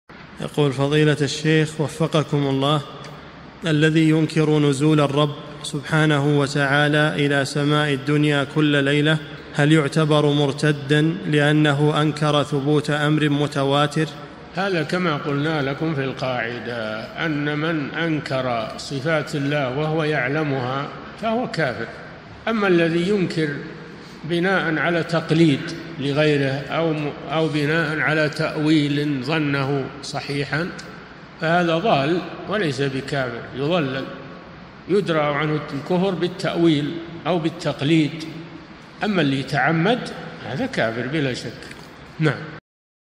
[383 -1024] هل يكفر من أنكر نزول الرب سبحانه وتعالى إلى سماء الدنيا كل ليلة؟ - الشيخ صالح الفوزان
-[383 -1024] السؤال: الذي ينكر نزول الرب سبحانه وتعالى إلى سماء الدنيا كل ليلة، هل يعتبر مرتدا لأنه أنكر ثبوت أمر متواتر ؟
الجواب: هذا كما قلنا لكم في القاعدة أن من أنكر صفات الله وهو يعلمها فهو كافر، أما الذي ينكر بناء على تقليد لغيره أو بناء على تأويل ظنه صحيحًا فهذا ضال وليس بكافر، يضلل يدرأ عنه الكفر بالتأويل أو بالتقليد أما الذي يتعمد فهذا كافر بلا شك.
للاشتراك في قناة الشيخ صالح الفوزان
سؤال وجواب من شرح الفتوى الحموية ( 1024 ) مقطع

للاستفسار والإقتراحات والدعم
رئيس الفريق
لمشروع كبار العلماء